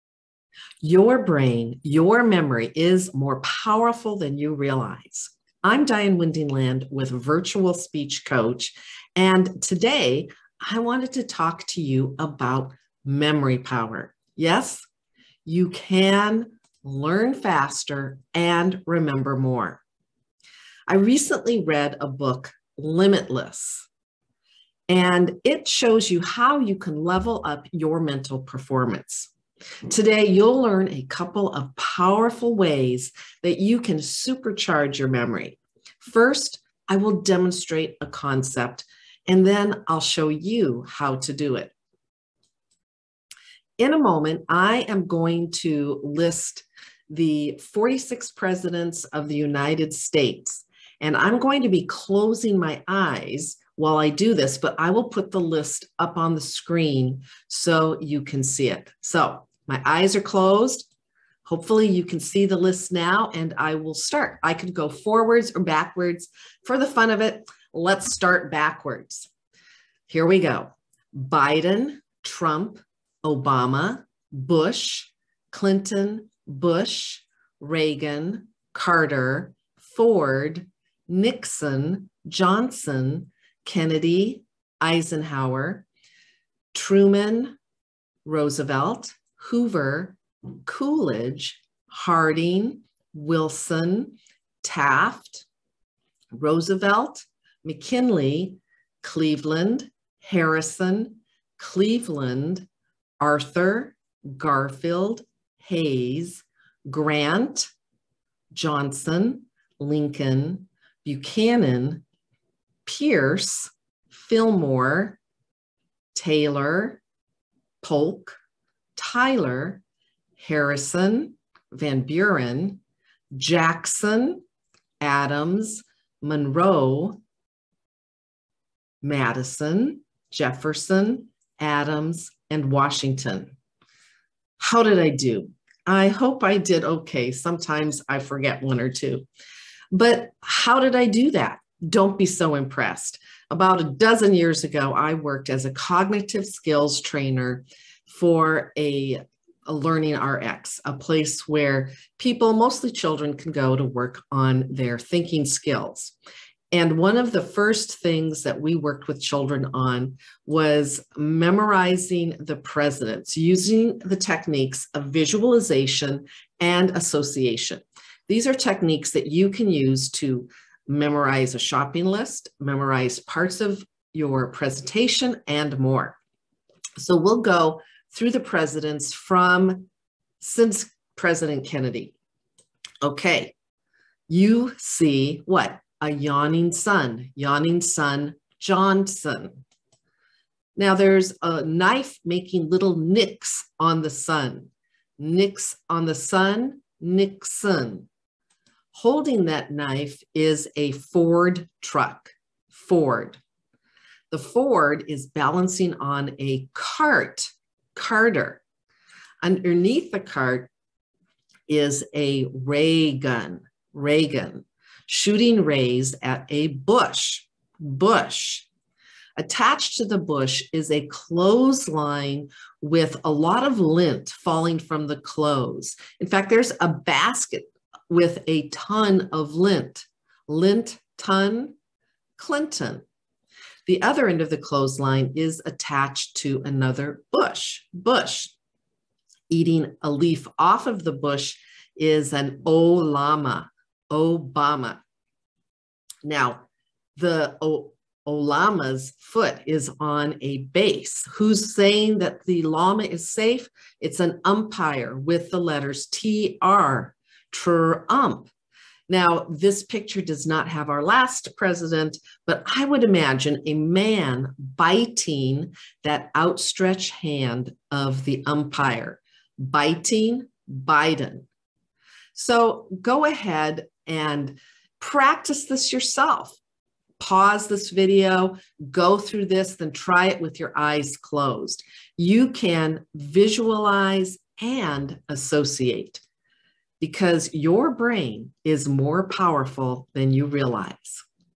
Increase your Memory: Visualization & Association
In this video, I demonstrate the techniques of visualization & association as I recite the list of 46 U.S. Presidents, from memory . . . backwards. This is a technique I originally learned years ago as a cognitive skills trainer for a Learning Rx franchise and revisited recently when I read the book, Limitless: Upgrade Your Brain, Learn Anything Faster, and Unlock Your Exceptional Life.